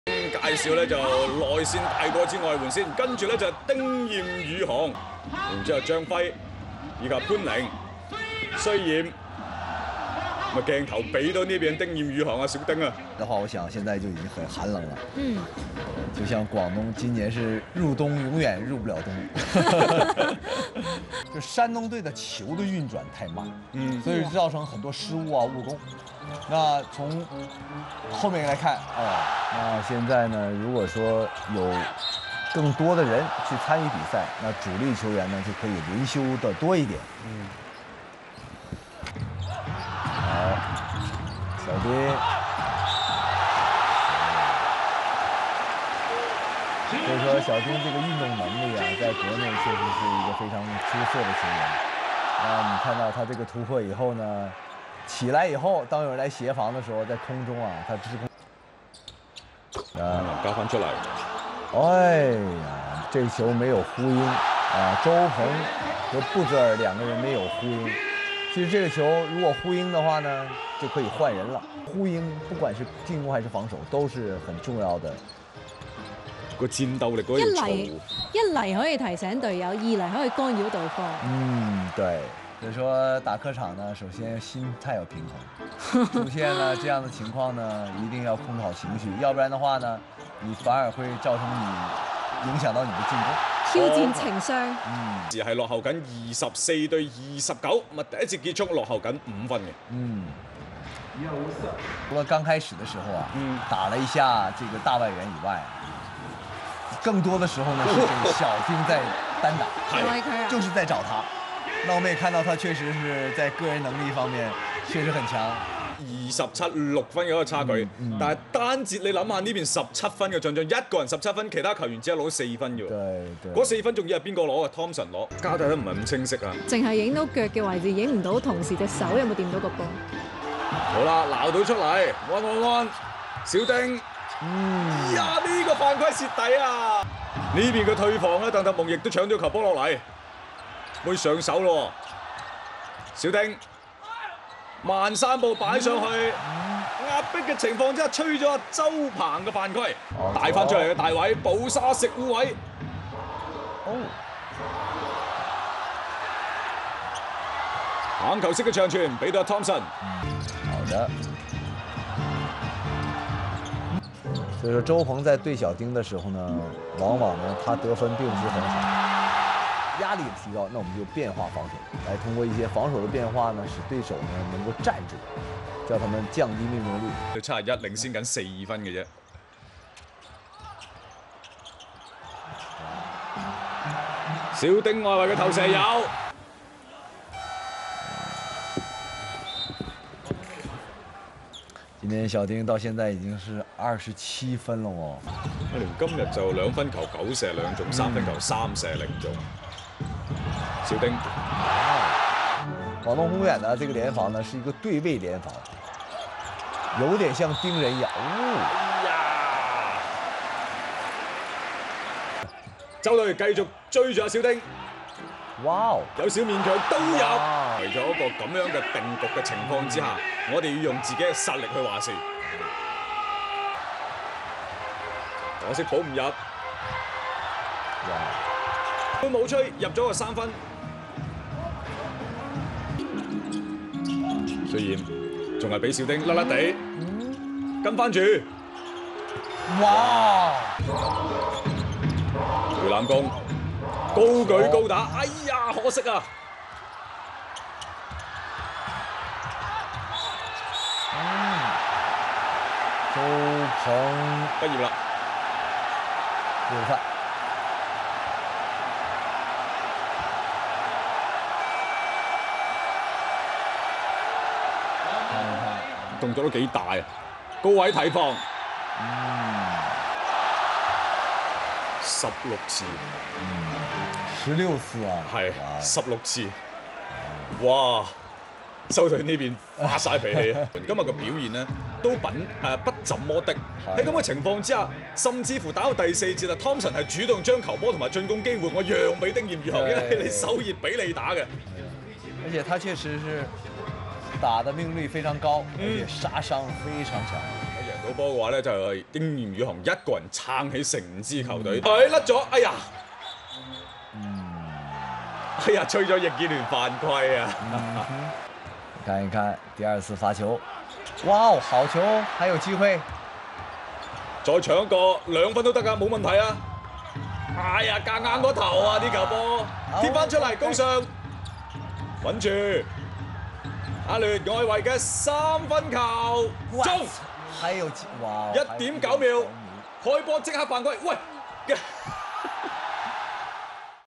丁彦雨航 (Ding Yanyuhang) 43 Pts Full Highlights vs 广东 (22.01.17) League-Bound? [1080p]
Does he have what it takes to make it to the NBA???

丁有能力立足nba吗？？？

77我好喜歡妳